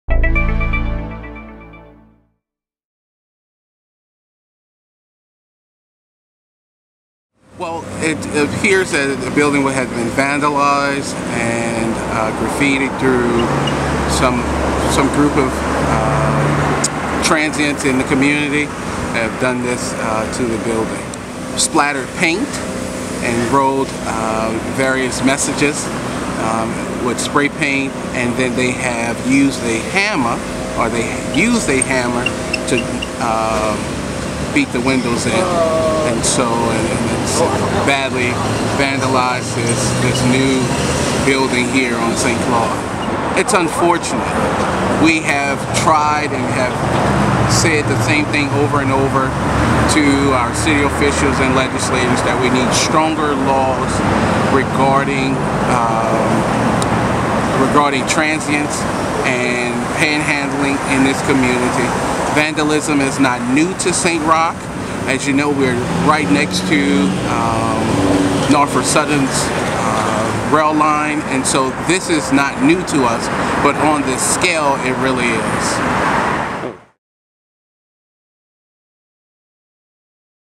Vandals strike St. Roch Market: video
Derrick Floyd, president of the Faubourg St. Roch Improvement Association, talks about the damage done by vandals who struck the St. Roch Market in the early morning hours of Friday, May 1, 2015.